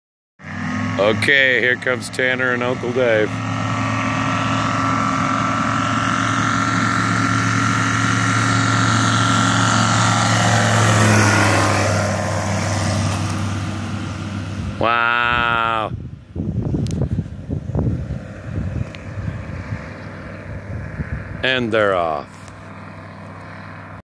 Zenith CH-701
Zenith CH-701 take off, 800 foot strip at 5000 feet MSL